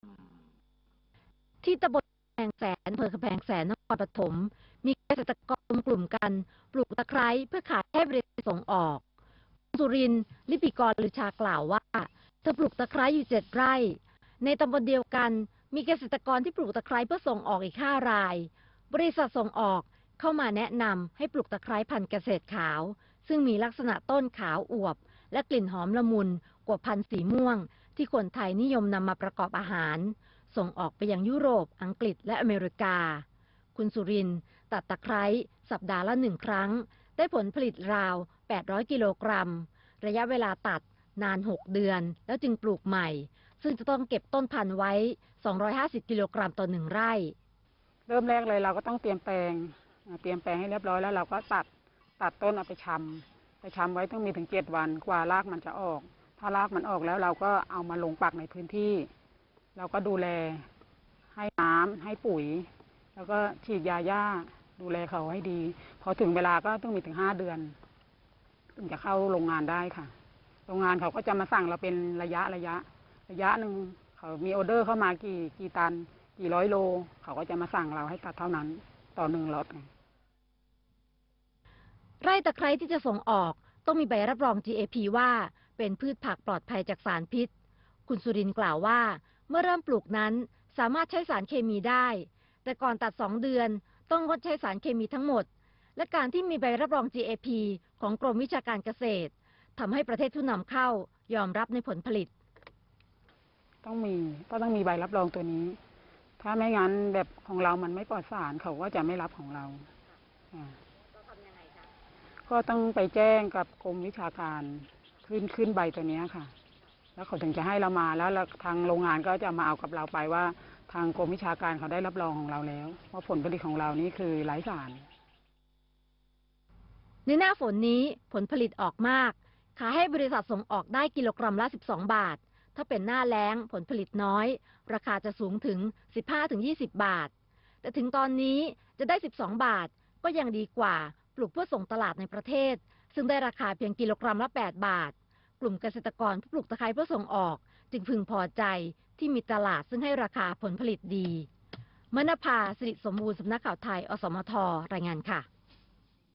เกษตรสร้างชาติ : เกษตรกรกำแพงแสน จ.นครปฐม ปลูกตะไคร้ส่งออก
นครปฐม 9 พ.ย.-กลุ่มเกษตรกรผู้ปลูกตะไคร้ใน อ.กำแพงแสน จ.นครปฐม สามารถผลิตตะไคร้ได้อย่างมีคุณภาพ มีบริษัทส่งออกมาทำสัญญารับซื้อ จึงมีรายได้ดีกว่าการปลูกตะไคร้ส่งตลาดในประเทศ.-สำนักข่าวไทย
1.สารคดีเฉลิมพระเกียรติ
2.พระราชกรณียกิจ-พระมหากรุณาธิคุณ
3.คนไทยหัวใจ♥รักในหลวง